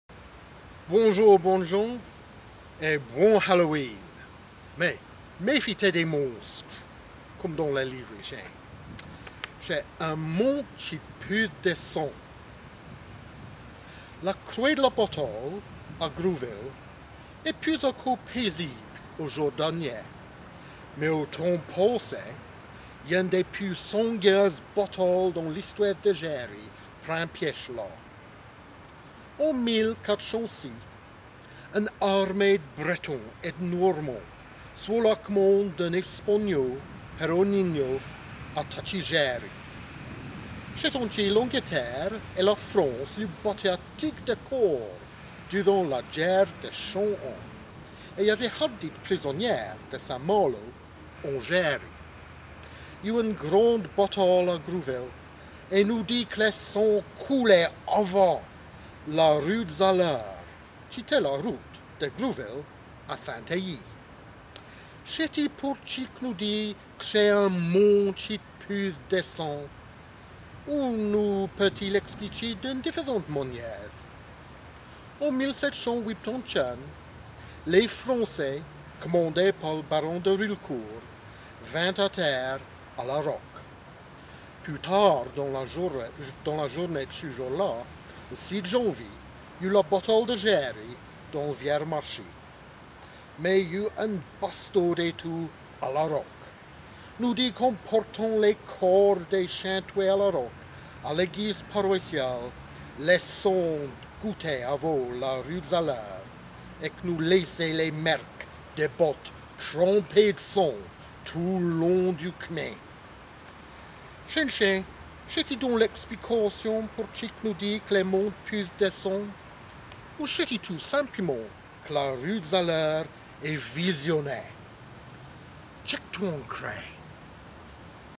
Un mont tchi d'puthe dé sang
"Un mont tchi d'puthe dé sang" - eune histouaithe du livret "Mêfie-té des monstres!"

The Jersey legend of Blood Hill.